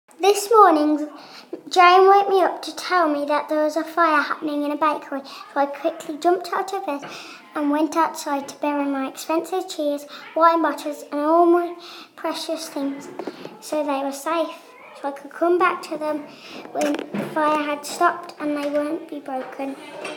Samuel Pepys
Today in Computing we used the 'Chatterpix' app to record the facts we have learnt about Samuel Pepys.